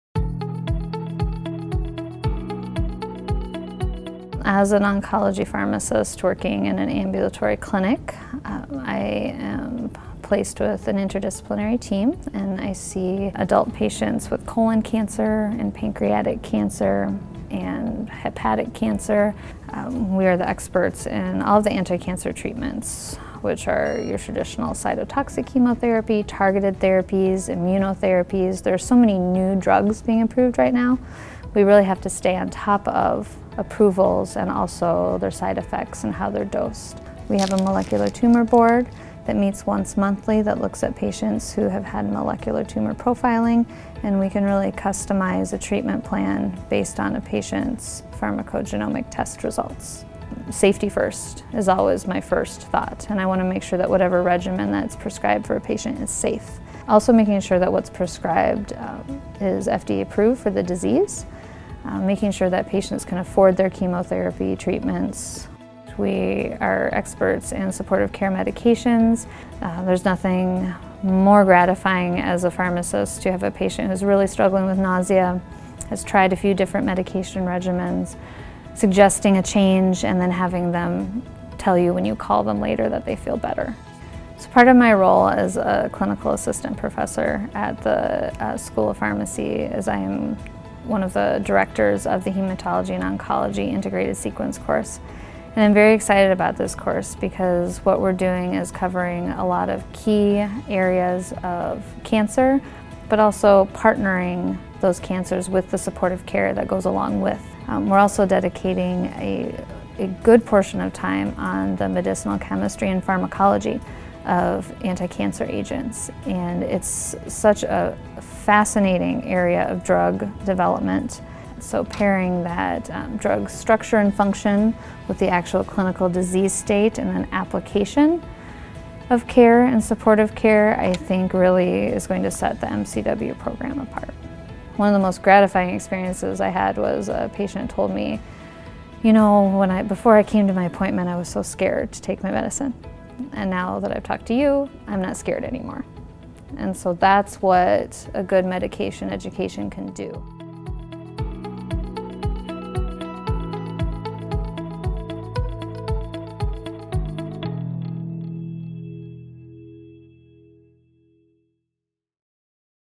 Pharmacist of the Future: Hematology/Oncology Pharmacist
At the Medical College of Wisconsin (MCW) School of Pharmacy, our innovative 3-year program provides early and extensive exposure to a variety of clinical settings and world-class research in specialties such as hematology and oncology. Carrie Oxencis, PharmD, BCPS, BCOP talks about her role as an Oncology Clinical Pharmacist and how she contributes to high-quality healthcare in an ambulatory care setting.